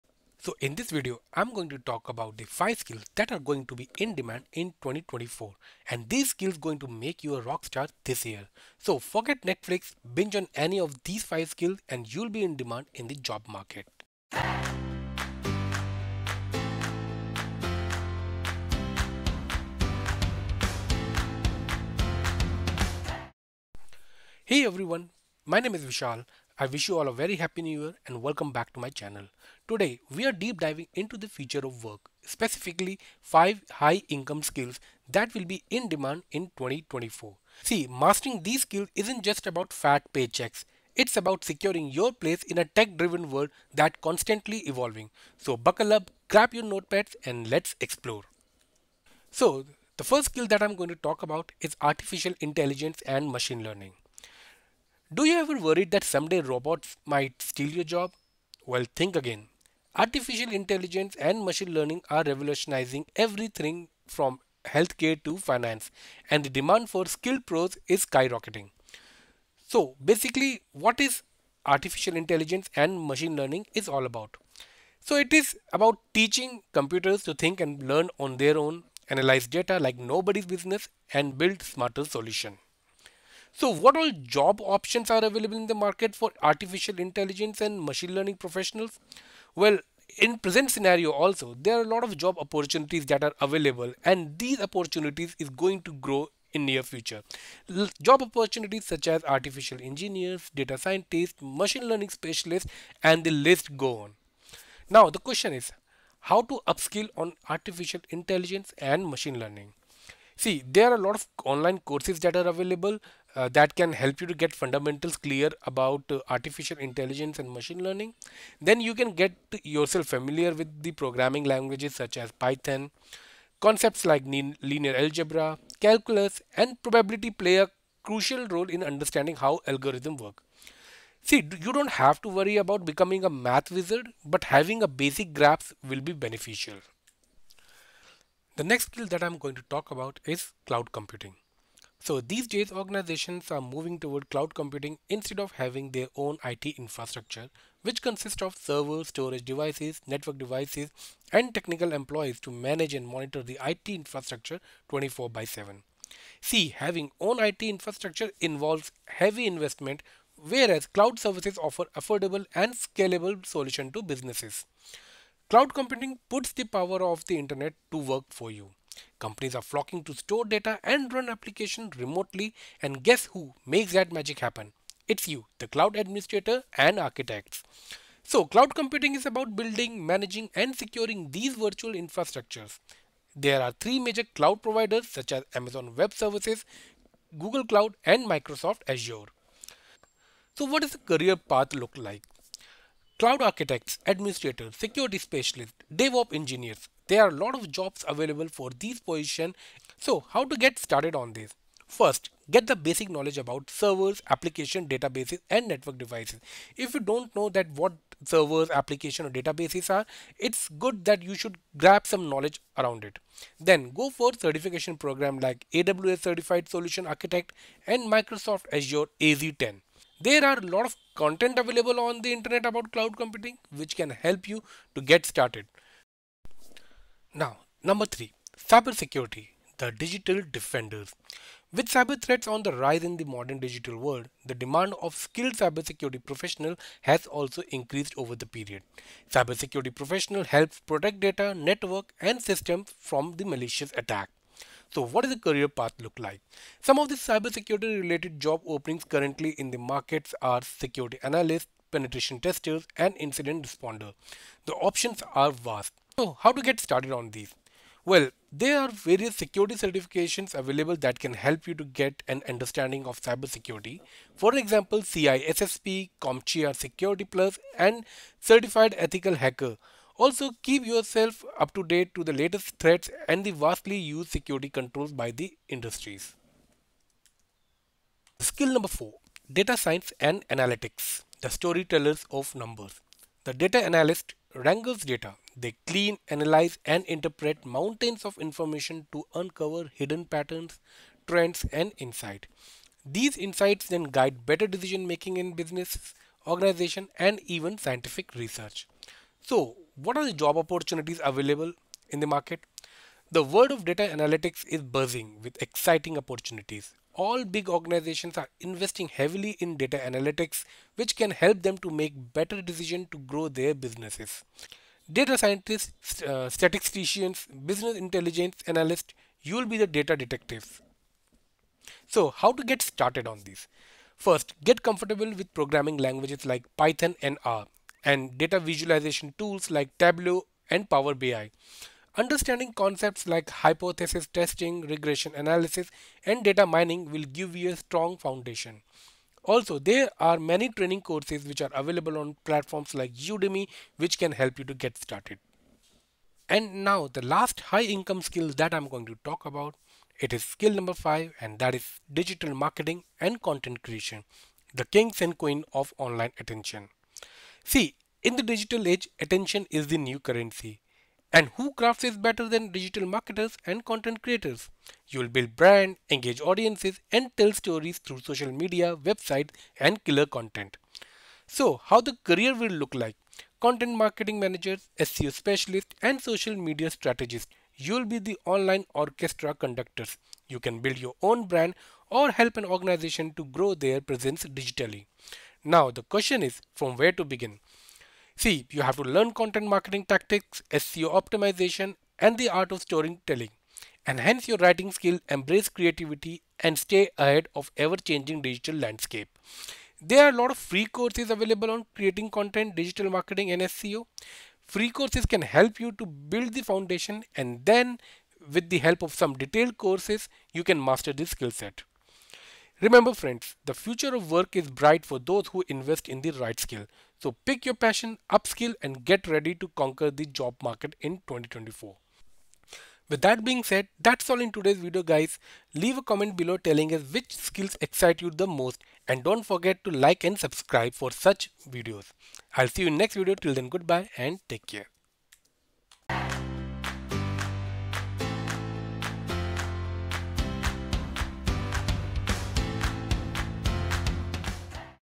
Top 5 High Income Skills in 2024..
In this video, I have covered the Top 5 High-Income Skills that will be in demand this year - 2024. These skills are
1. Artificial Intelligence & Machine Learning
2. Cloud Computing
3. Cybersecurity
4. Data Science & Analytics
5. Digital Marketing & Content Creation

✅ Read my latest Article:
➡️ Browsing the Web Safely: The Importance of HTTPS